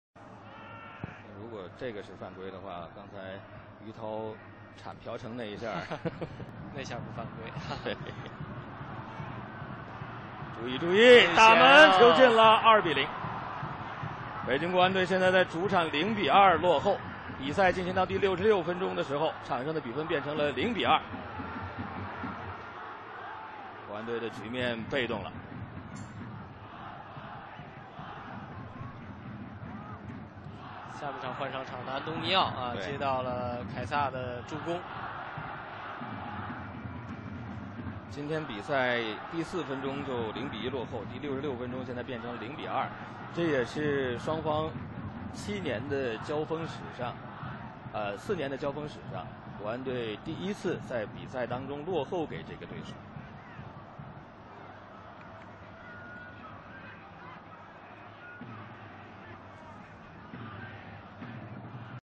Antonio Flavio Goal 2013.4.14 5th ROUND 2013 CSL Beijing Guoan 2:2SHANGHAI
Kieza assit Antonio Flavio at 66 min